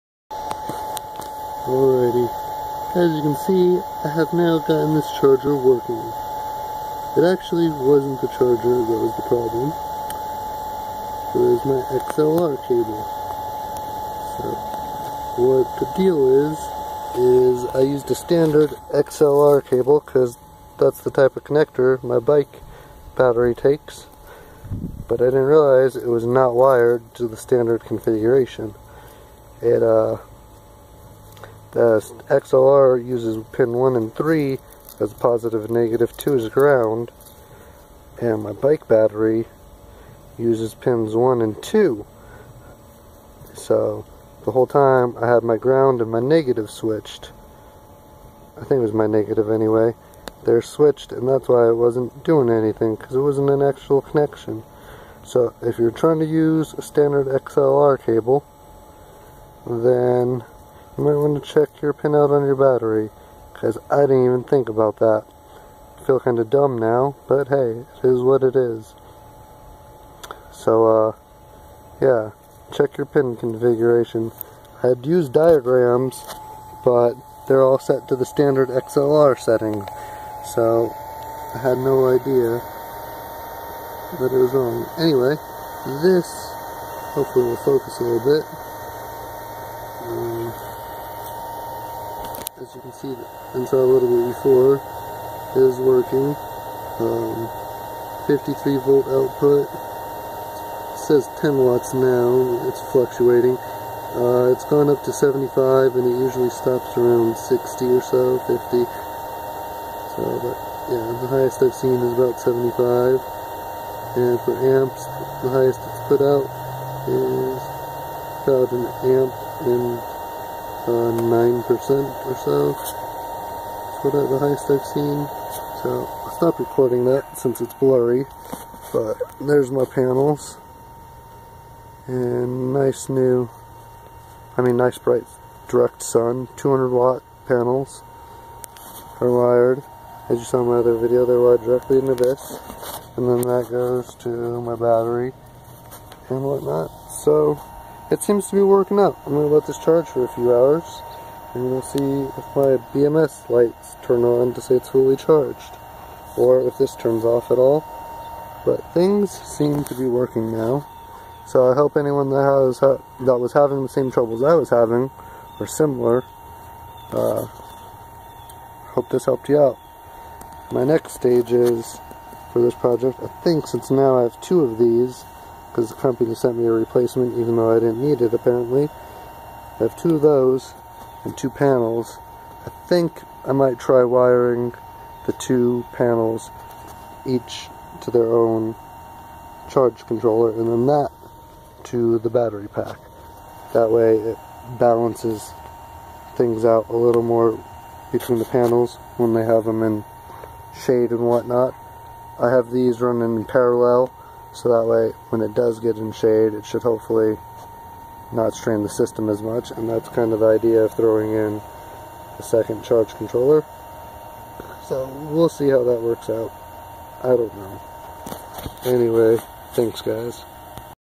Solar Charging Problem SOLVED!!!
Fixed the problem with my solar charging system. its working now!